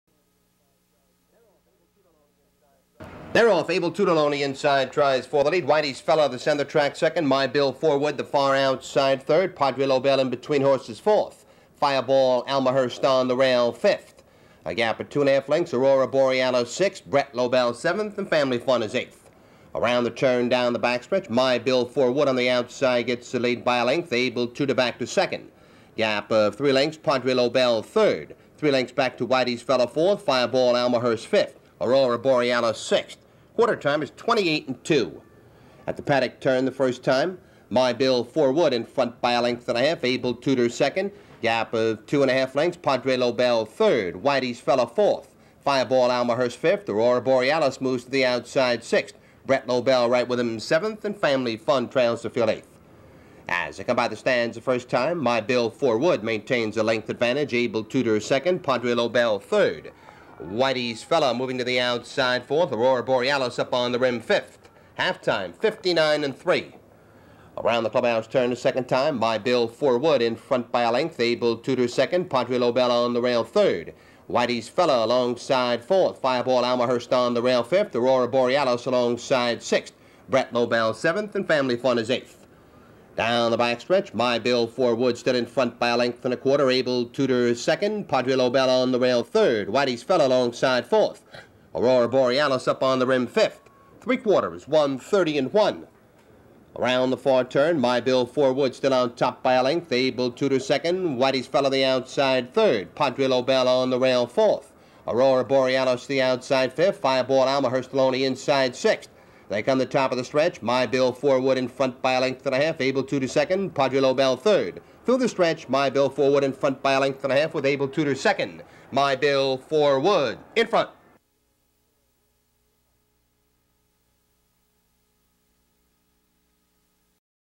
1984 Yonkers Raceway - My Bill Forwood & John Patterson Jr
Open Pace 35,000
1- Able Tudor
2- Padre Lobell
3- Fireball Almahurst
4- Whiteys Fella
5- My Bill Forwood
6- Aurora Borealis
7- Bret Lobell
8- Family Fun N